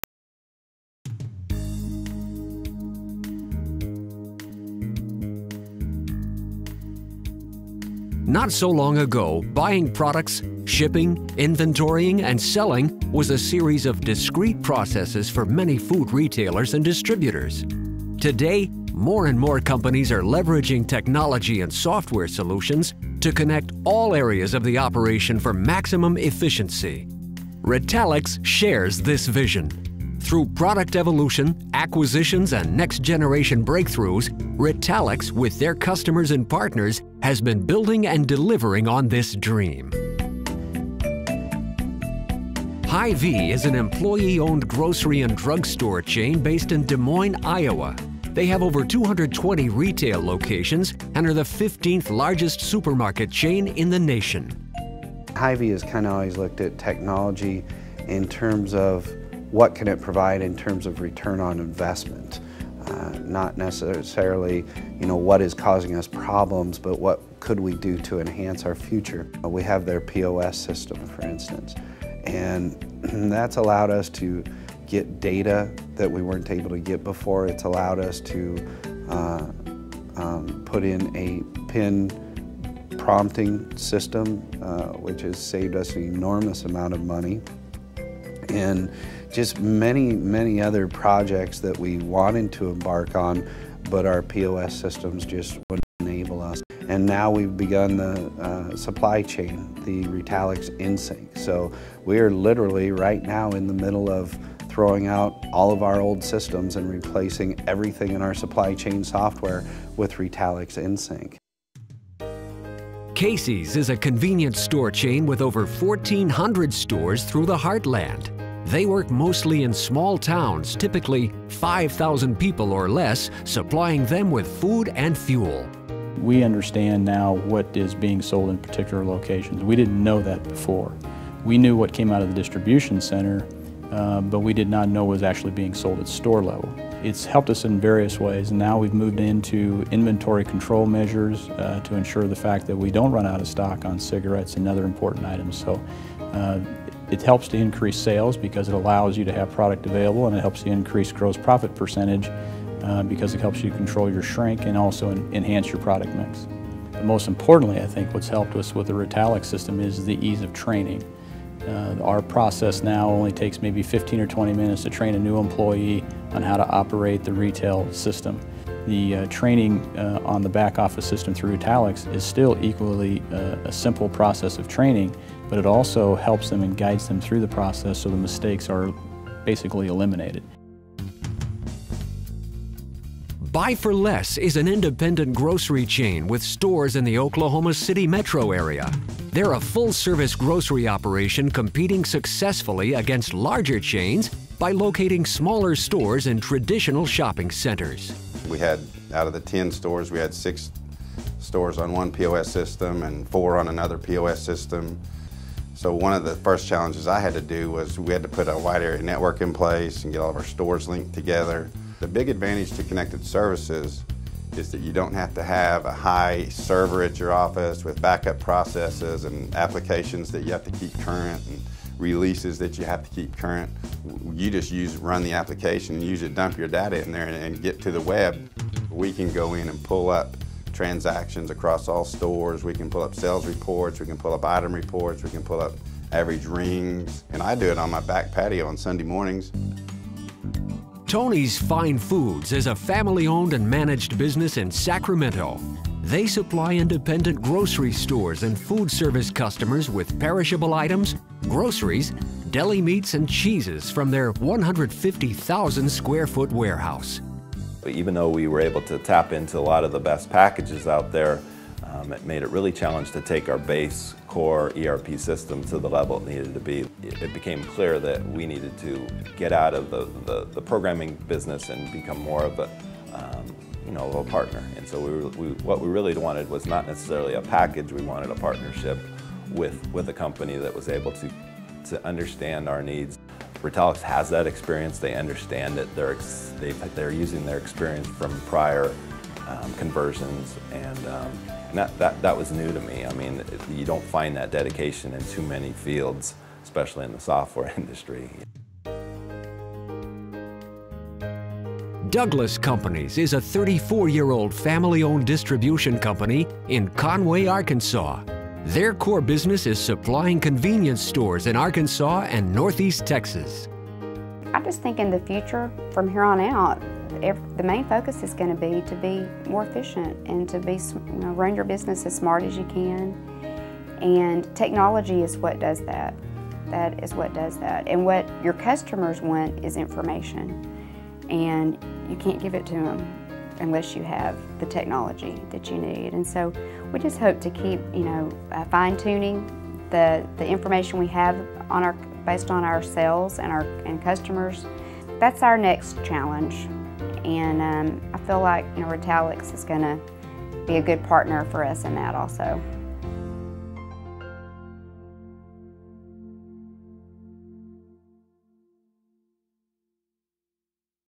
Retalix Customer Testimonials
Retalix USA customers - grocery, convenience and food service retailers and distributors - discuss their business challenges and how information technology helps them.